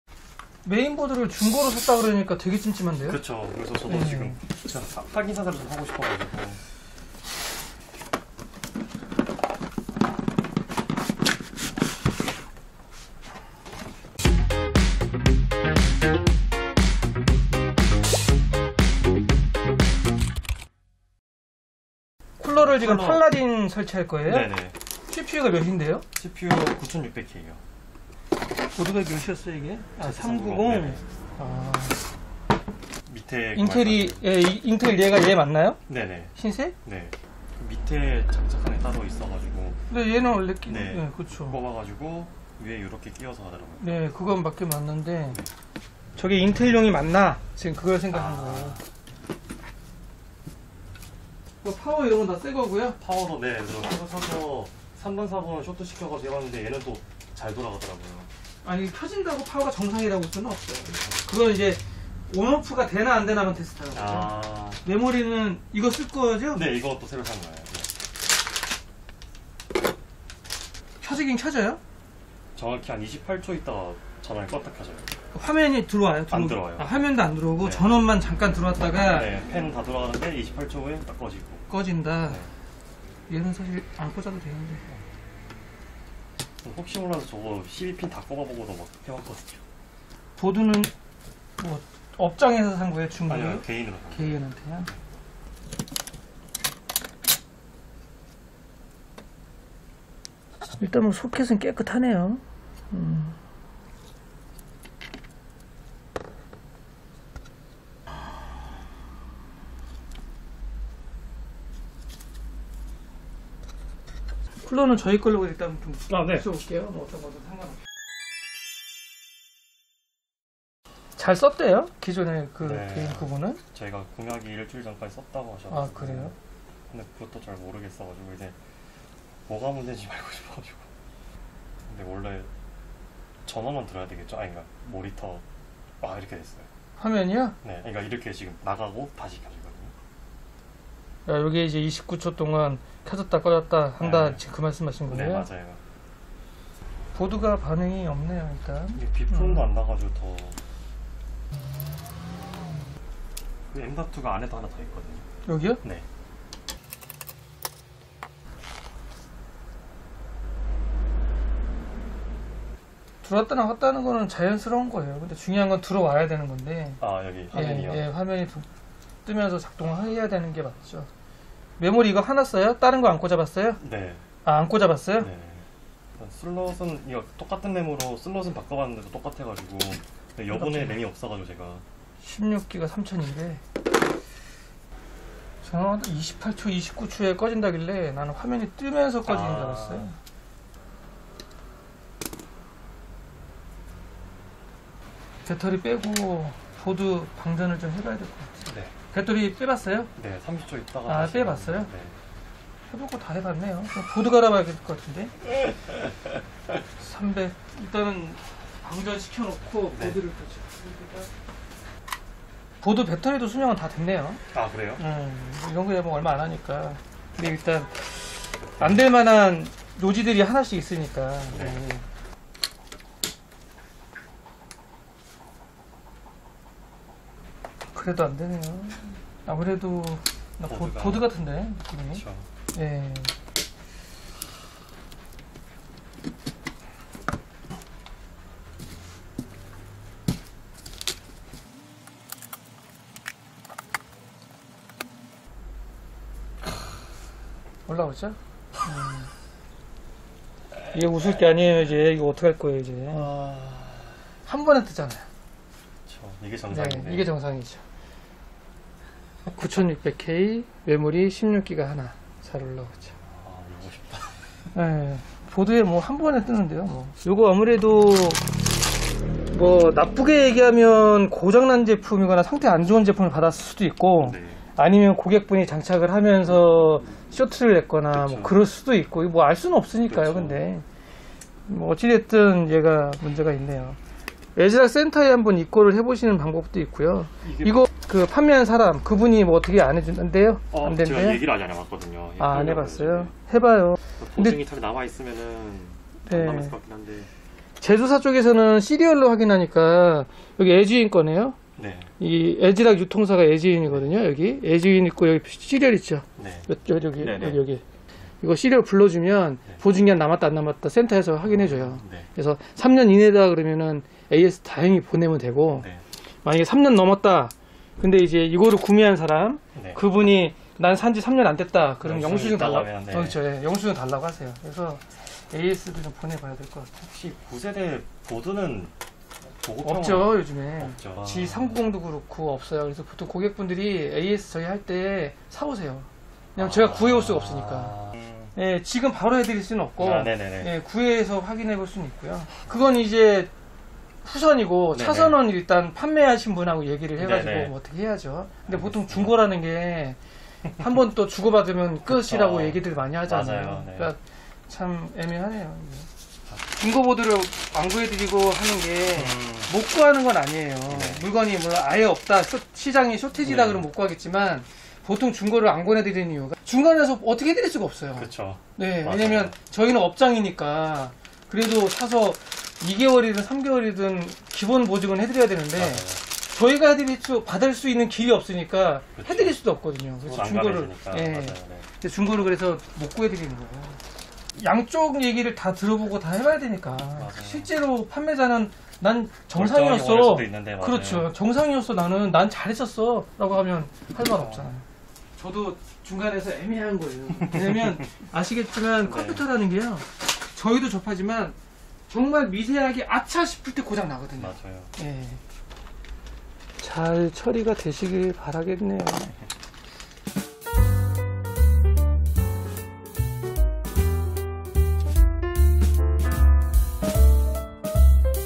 ASRock Z390 익스트림4 메인보드 28초 후에 껐다 켜집니다 | 중고거래 | 컴퓨터 매장 수리 일상
감사합니다 ❤😊🎉